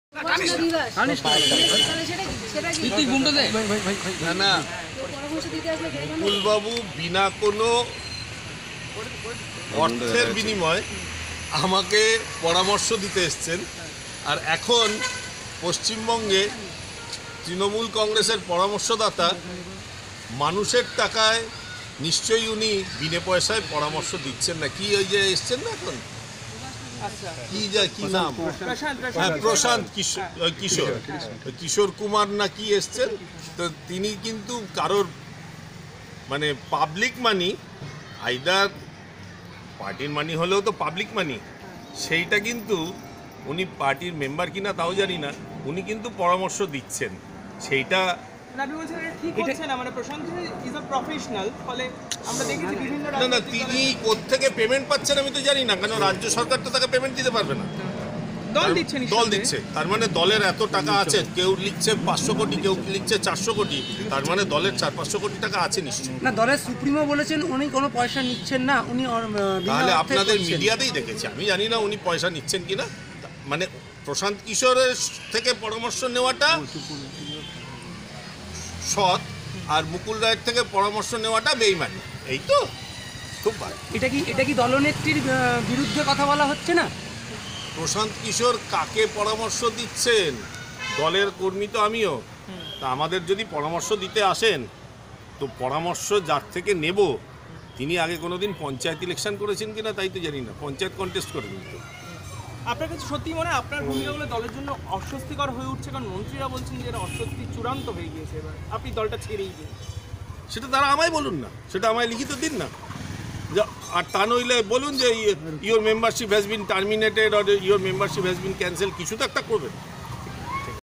Sabyasachi Dutta
Sabyasachi Dutta of Bidhannagar Mayor giving bite after Mukul Roy meet and reaction of Trinamool